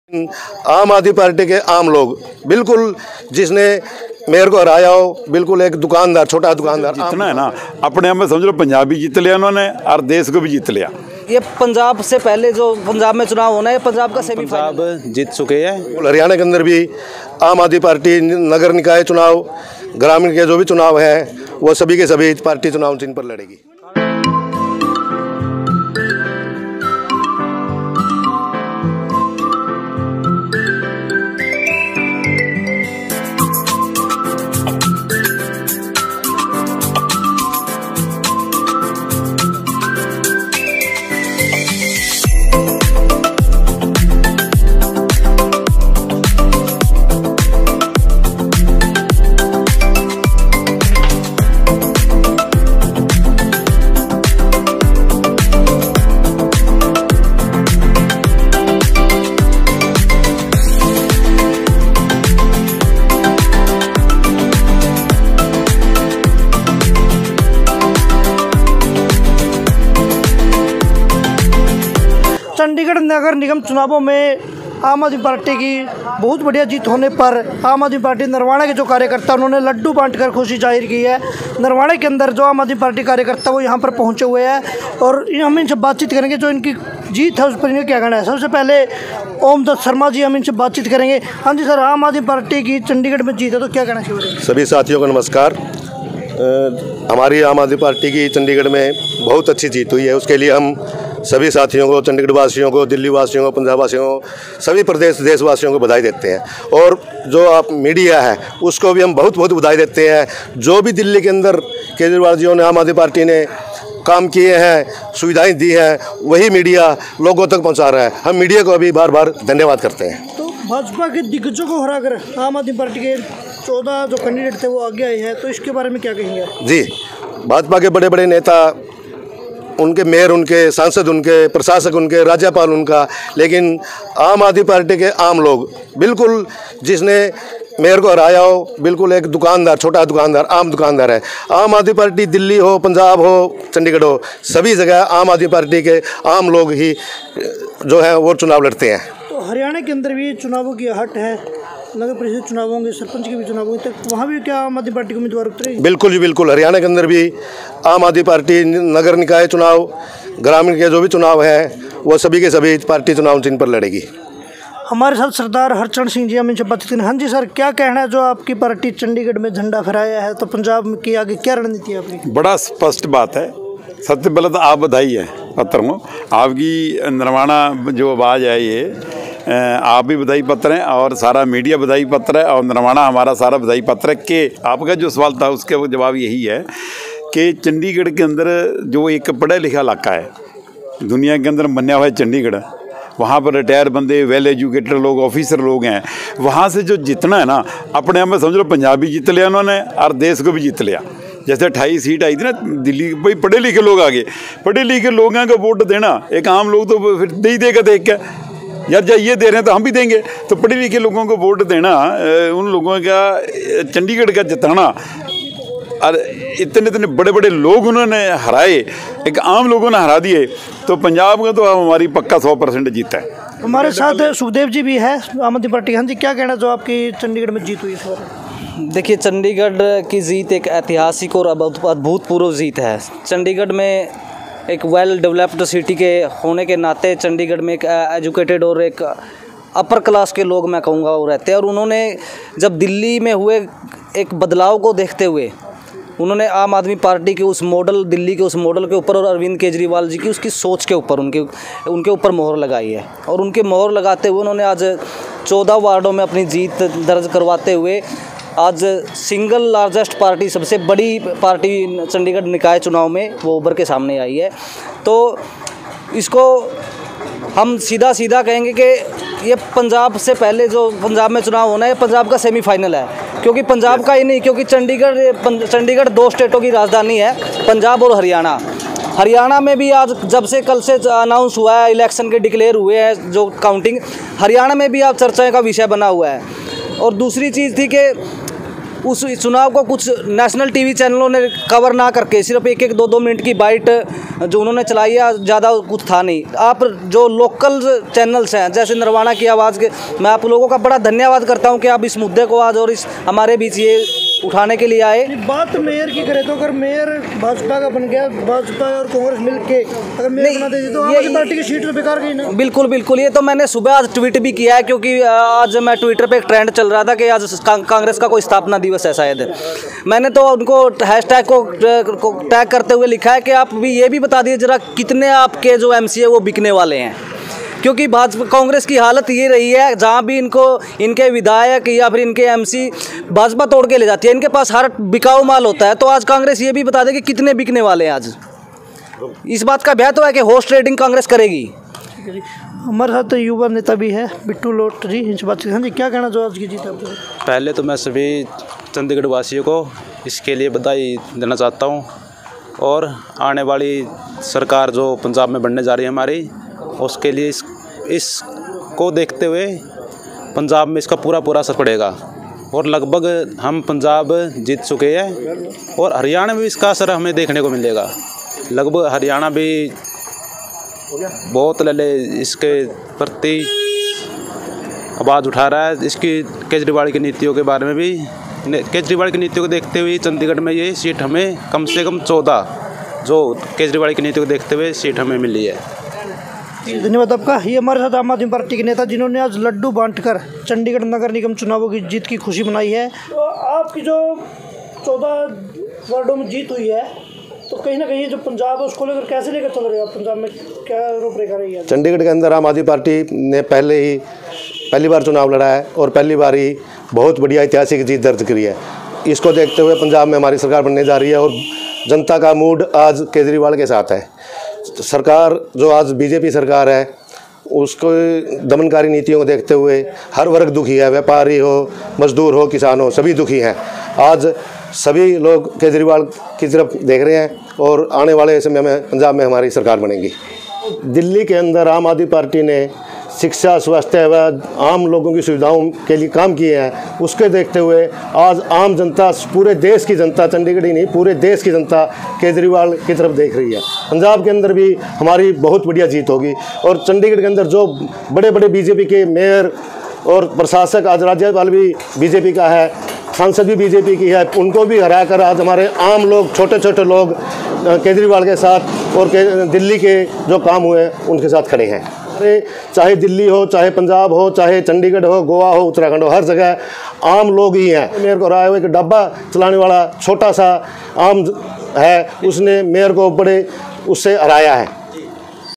आप कार्यकर्ताओं ने बांटे लड्डू, चंडीगढ़ जीत ली अब पंजाब और हरियाणा की बारी
narwana aap aap_party chandigarh आप कार्यकर्ताओं ने बांटे लड्डू, चंडीगढ़ जीत ली अब पंजाब और हरियाणा की बारी